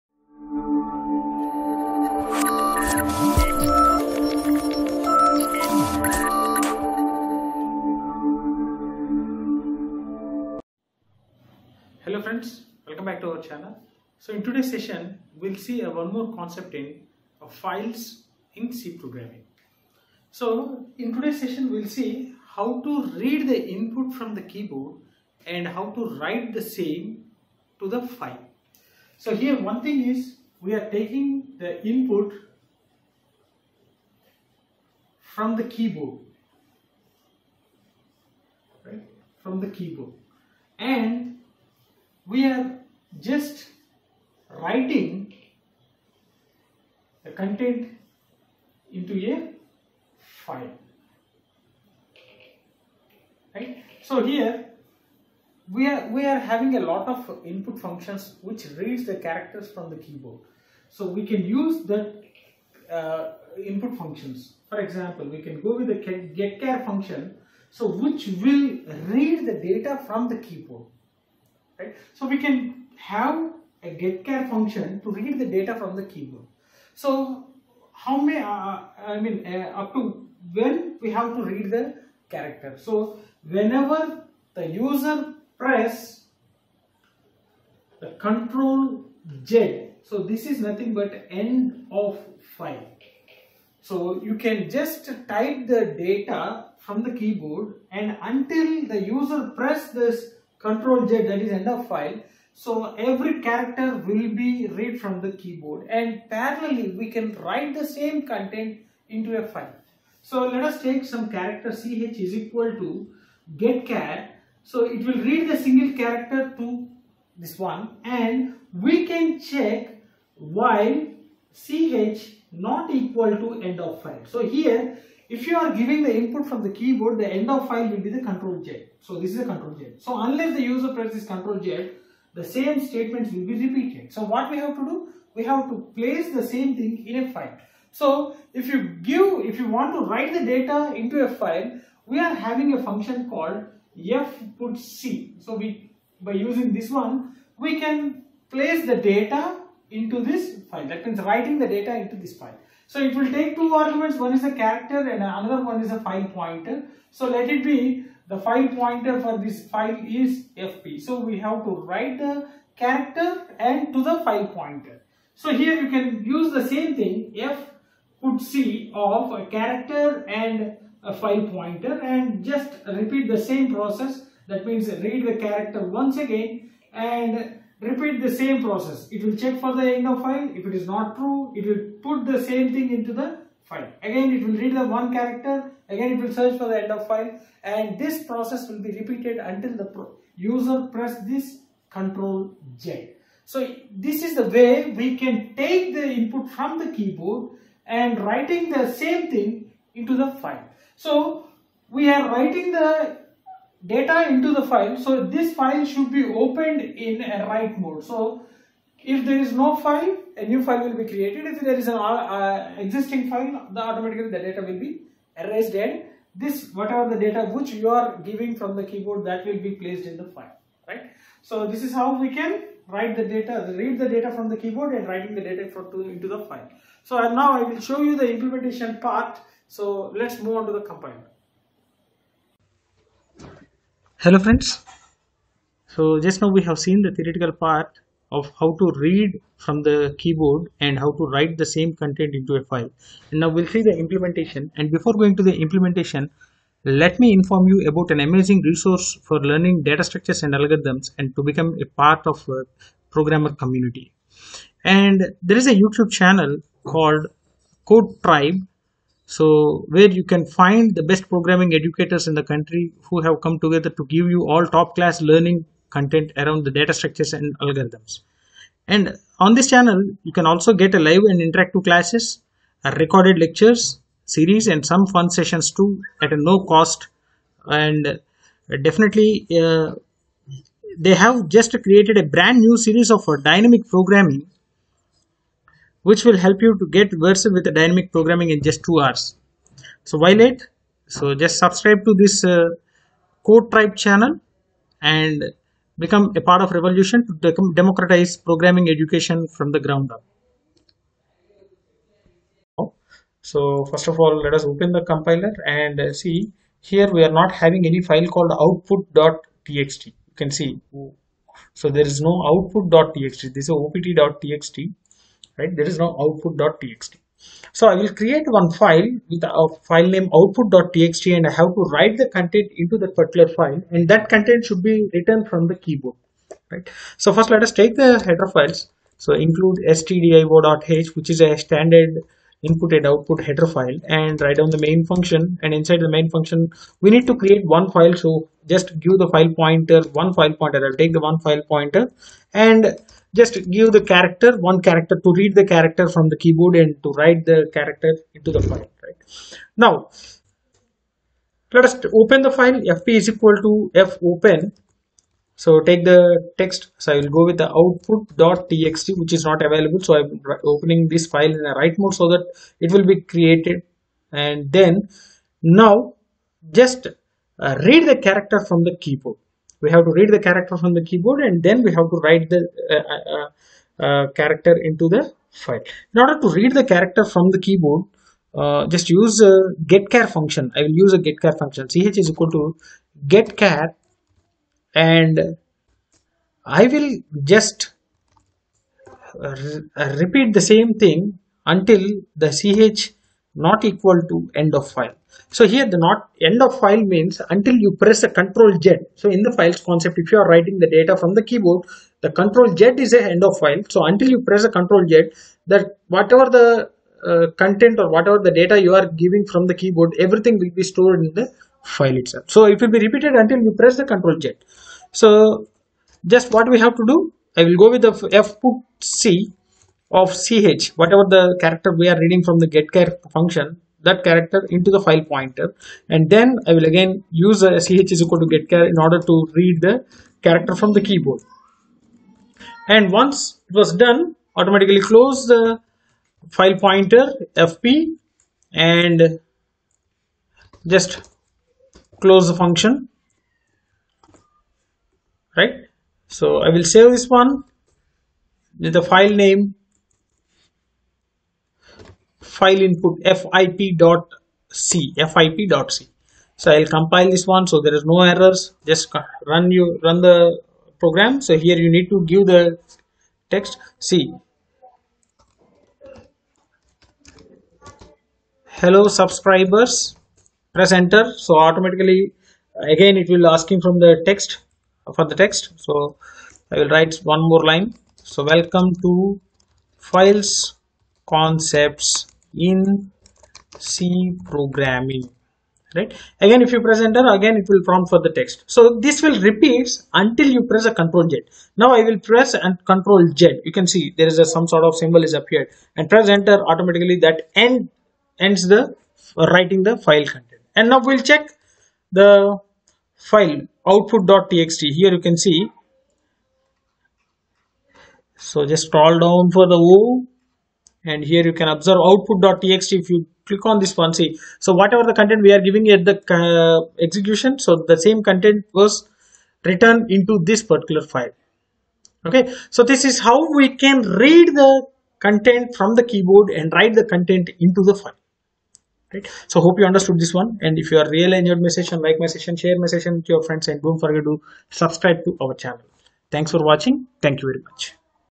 READ INPUT FROM KEYBOARD AND WRITE INTO A FILE IN C PROGRAMMING || READING AND WRITING IN FILES
list=PLwj0XzmRlS9kjnQng6jQE9WwJPJ4qrBIr

fgetc( )  is used to read the data from the file
syntax : Variable = fgetc(file_Pointer)
fputc( ) is used to write the data into the file.
syntax : fputc(variable , file_Pointer)